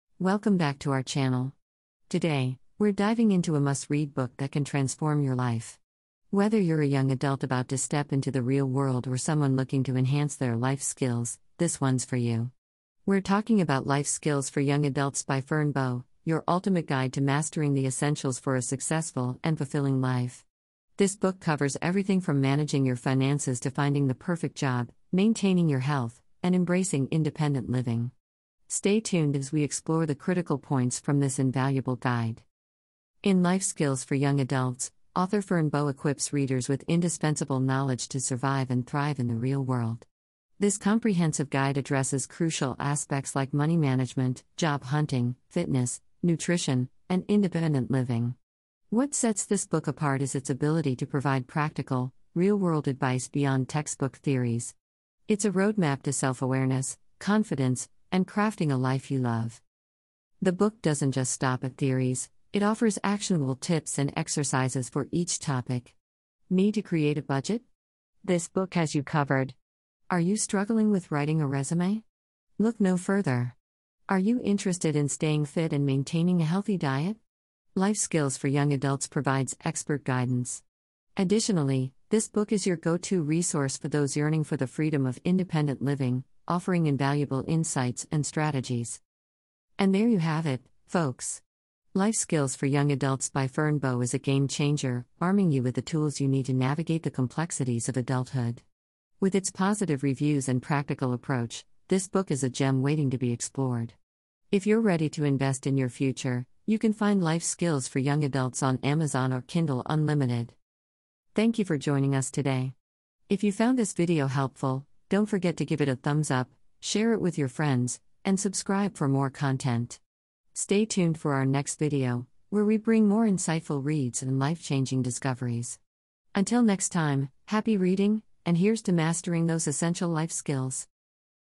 The Ultimate Life Skills Guide: Ferne Bowe's Expert Review For Thriving Young Adults
Are you looking for a book that can teach you the essential life skills you need to succeed in adulthood?
If so, please check out my Life Skills for Young Adults review by Ferne Bowe.
This book is a comprehensive guide to mastering money management, job hunting, fitness, nutrition, and independent living skills.
It also helps you gain practical and real-world advice, self-awareness and confidence, and a life you love.
In this video, I’ll share with you some of the main takeaways from the book, as well as my personal opinion on why you should read it.
Whether you’re a teen, a young adult, or a parent, this book is for you.

Life Skills